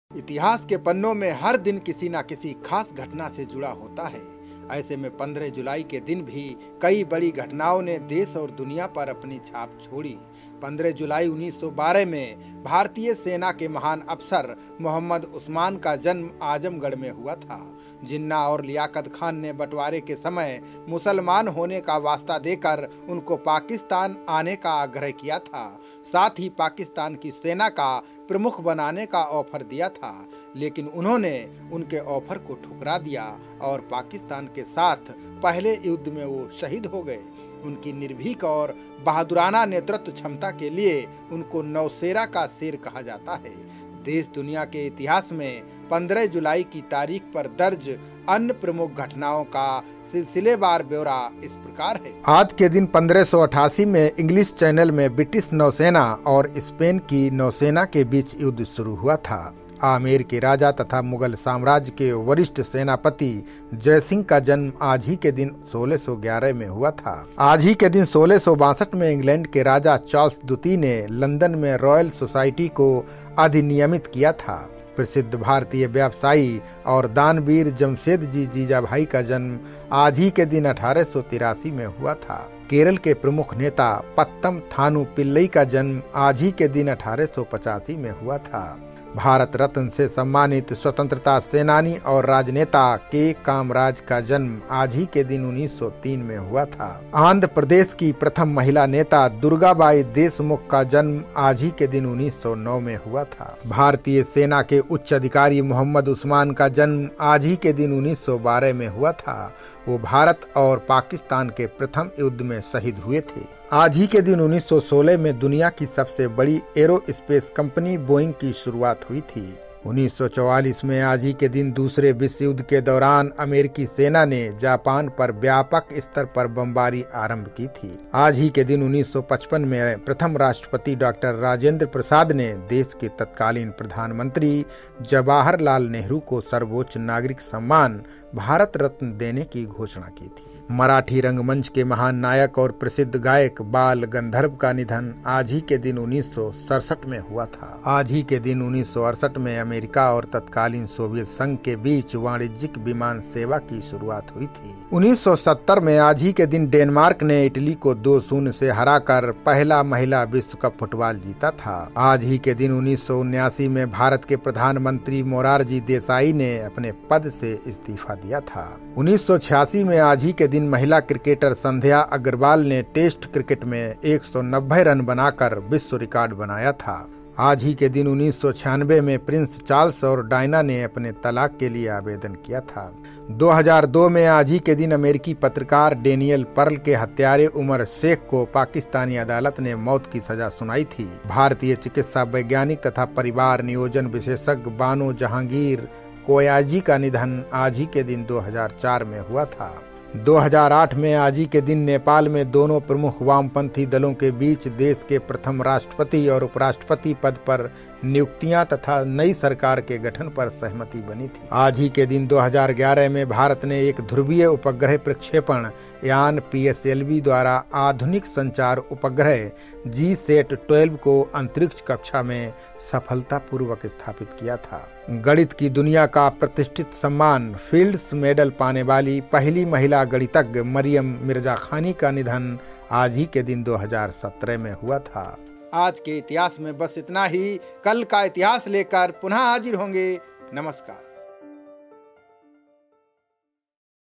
15-07-2021 | 15 जुलाई को घटने वाली बड़ी घटनाएं | Today History | Today Birthday Hindi News | Events
इतिहास के पन्नों में हर दिन किसी न किसी खास घटना से जुड़ा होता है। ऐसे में 15 जुलाई के दिन भी कई बड़ी घटनाओं ने देश और दुनिया पर अपनी छाप छोड़ी। 15 जुलाई 1912 में भारतीय सेना के महान अफसर मोहम्मद उस्मान का जन्म आजमगढ़ में हुआ था। जिन्ना और लियाकत खान ने बंटवारे के समय मुसलमान होने का वास्ता देकर उनको पाकिस्तान आने का आग्रह किया था, साथ ही पाकिस्तान की सेना का प्रमुख बनाने का आॅफर दिया था। लेकिन, उन्होंने उनके आॅफर को ठुकरा दिया और पाकिस्तान के साथ पहले युद्ध में वह शहीद हो गए। उनकी निर्भीक और बहादुराना नेतृत्व क्षमता के लिए उनको नौशेरा का शेर कहा जाता है। देश-दुनिया के इतिहास में 15 जुलाई की तारीख पर दर्ज अन्य प्रमुख घटनाओं का सिलसिलेवार ब्योरा इस प्रकार है।
1
आज के दिन 1588 में इंग्लिश चैनल में ब्रिटिश नौसेना और स्पेन की नौसेना के बीच युद्ध शुरू हुआ था।
2
आमेर के राजा तथा मुगल साम्राज्य के वरिष्ठ सेनापति जयसिंह का जन्म आज ही के दिन 1611 में हुआ था।
3
आज ही के दिन 1662 में इंग्लैंड के राजा चार्ल्स द्वितीय ने लंदन में रॉयल सोसायटी को अधिनियमित किया था।
4
प्रसिद्ध भारतीय व्यवसायी और दानवीर जमशेद जी जीजाभाई का जन्म आज ही के दिन 1883 में हुआ था।
5
केरल के प्रमुख नेता पत्तम थानु पिल्लई का जन्म आज ही के दिन 1885 में हुआ था।
6
भारत रत्न से सम्मानित स्वतंत्रता सेनानी और राजनेता के. कामराज का जन्म आज ही के दिन 1903 में हुआ था।
7
आंध्र प्रदेश की प्रथम महिला नेता दुर्गाबाई देशमुख का जन्म आज ही के दिन 1909 में हुआ था।
8
भारतीय सेना के उच्च अधिकारी मोहम्मद उस्मान का जन्म आज ही के दिन 1912 में हुआ था। वह भारत और पाकिस्तान के प्रथम युद्ध में शहीद हुए थे।
9
आज ही के दिन 1916 में दुनिया की सबसे बड़ी एयरोस्पेस कंपनी बोइंग की शुरूआत हुई थी।
10
1944 में आज ही के दिन दूसरे विश्व युद्ध के दौरान अमेरिकी सेना ने जापान पर व्यापक स्तर पर बमबारी आरंभ की थी।